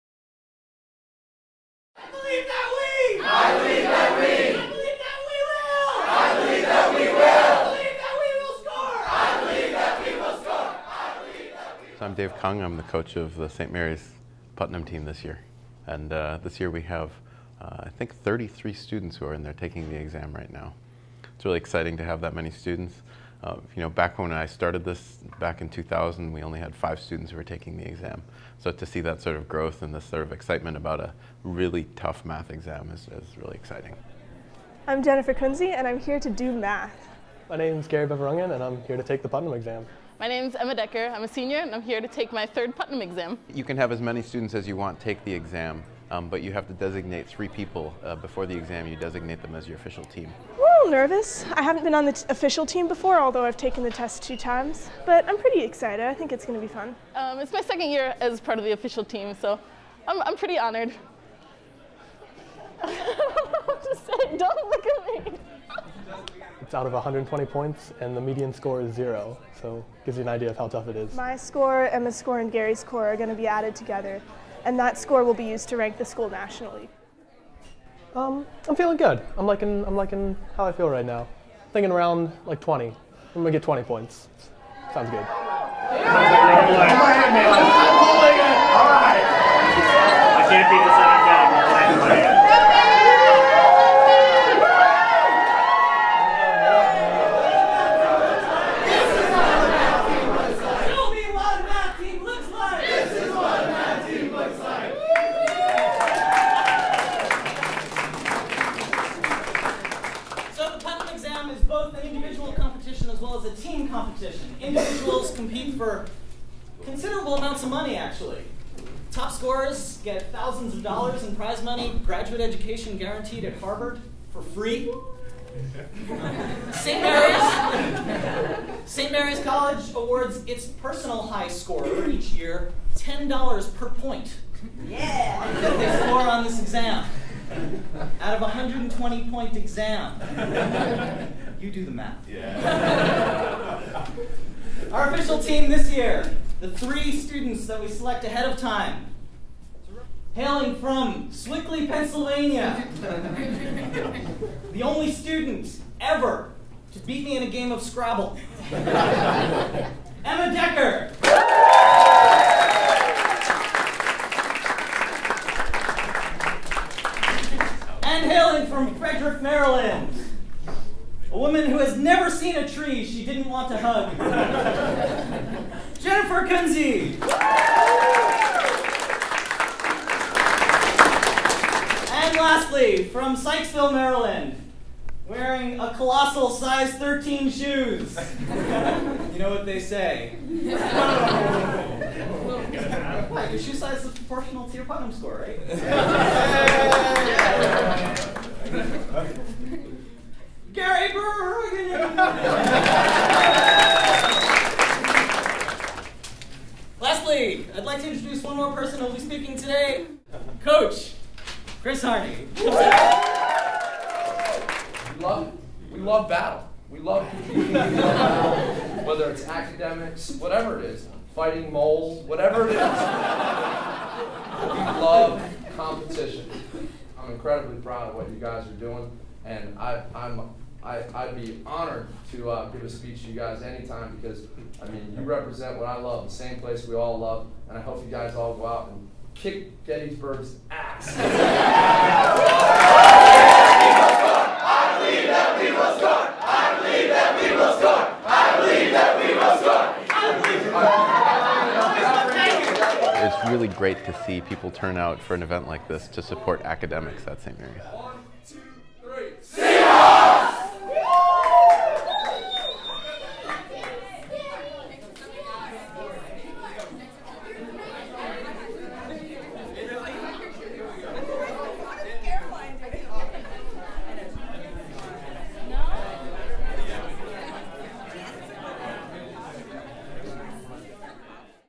2011 SMCM Putnam Exam Rally
The 30+ members of the St. Mary's College of Maryland Putnam Team prepare for and take the William Lowell Putnam Mathematical Exam, the hardest math competition out there for undergraduate math students. A boisterous crowd greets the students for a pep rally before they sit down for the six hour exam.